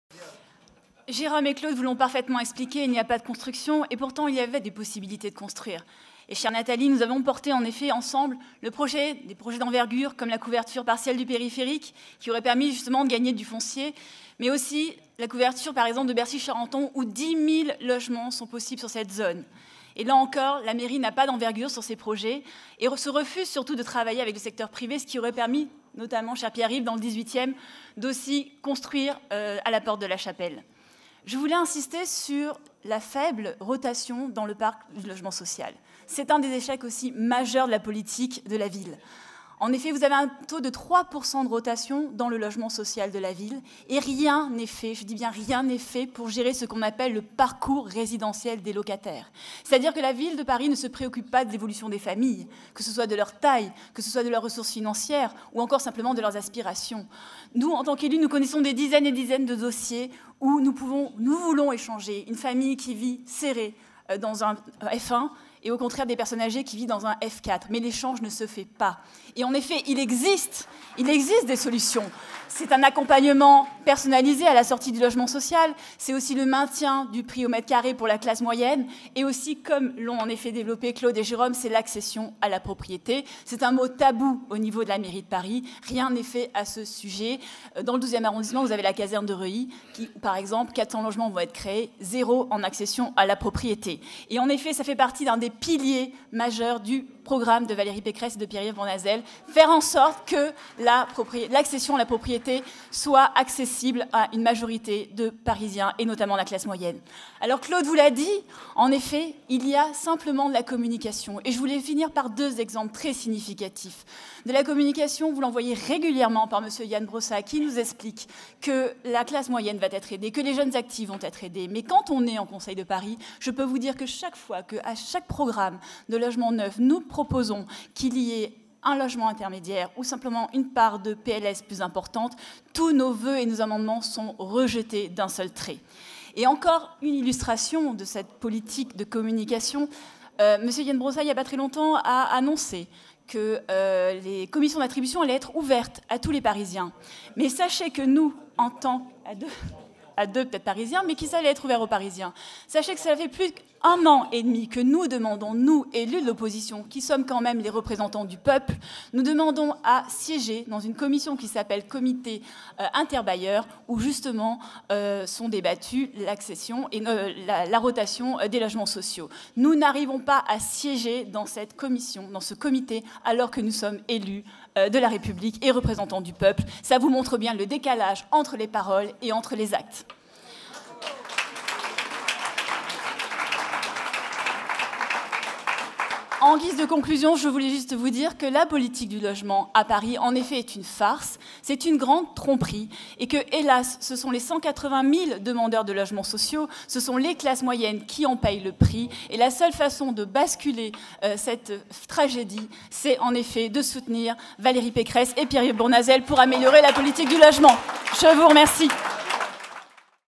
Intervention de Valérie MONTANDON sur les problèmes de logement à Paris - Etats généraux - 10/10/15
Valérie MONTANDON, Conseillère de Paris, intervient sur les problèmes de logement à Paris lors des Etats généraux de la Fédération de Paris des Républicains dans le cadre de la table ronde « Le logement à Paris » - 10 octobre 2015.